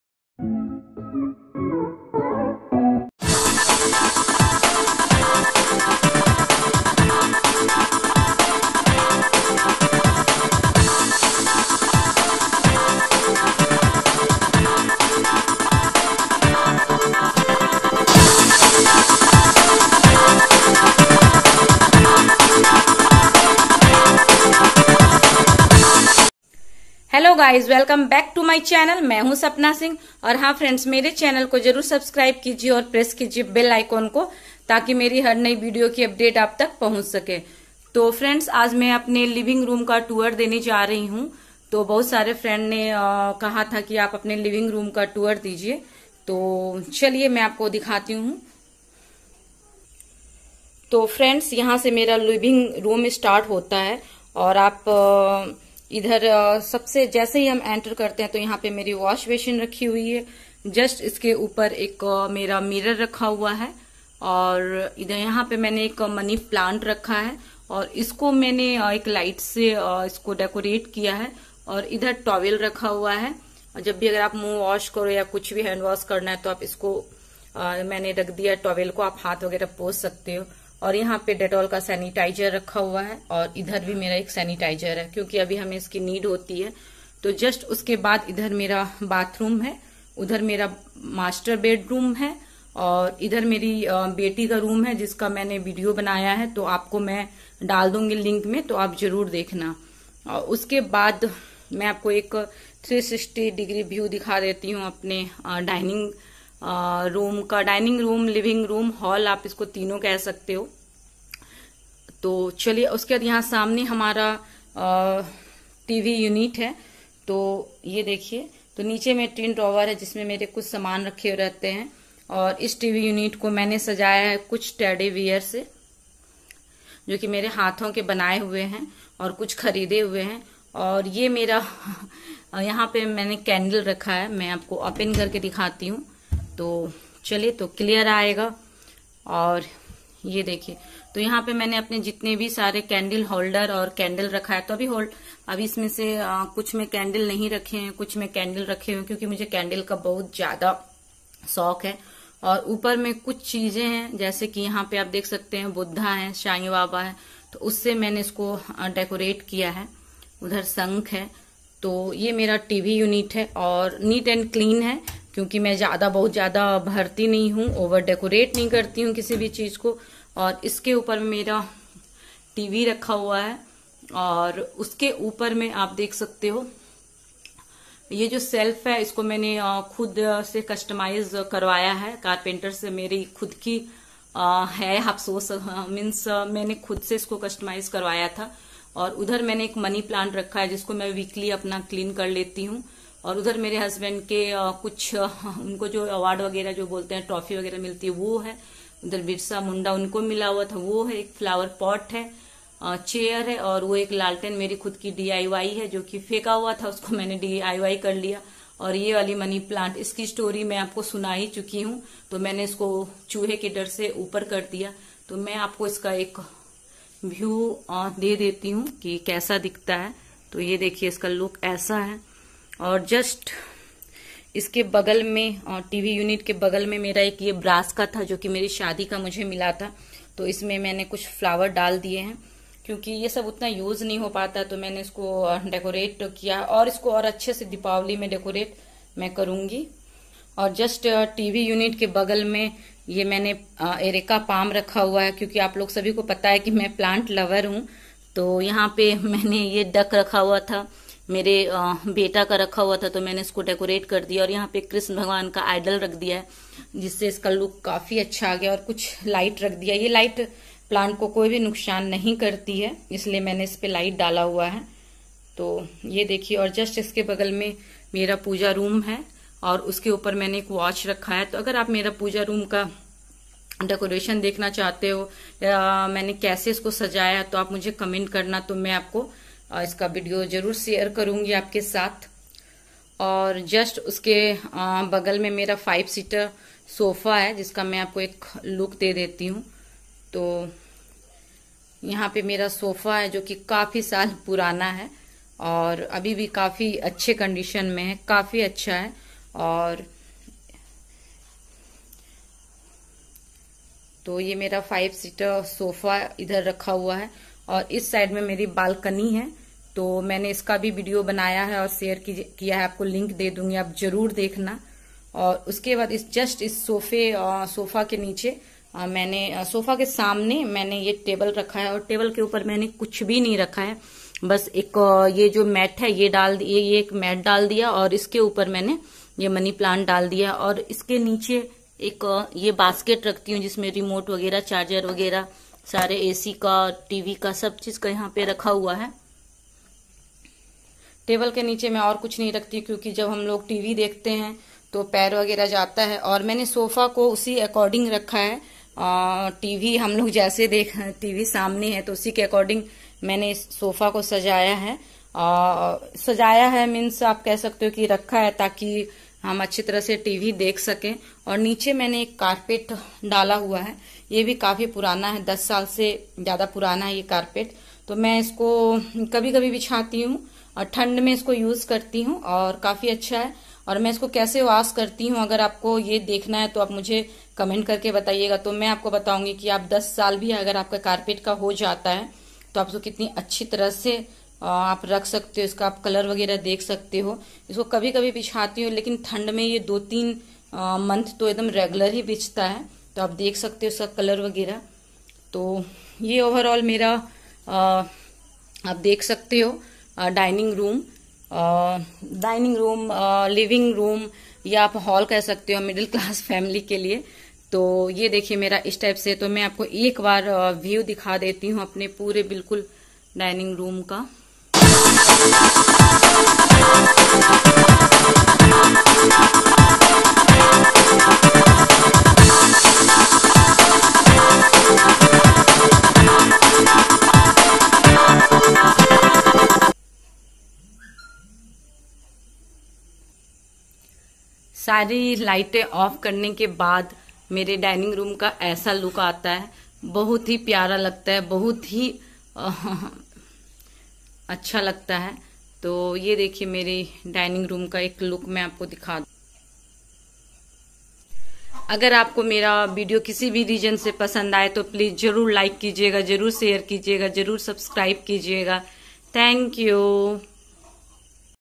Small Indian Living Room Tour//
ye video Maine apne living room ko kaise dacorete kiya hai usi ke  bare mai hai.

Following with the ding dong bell icon 🔔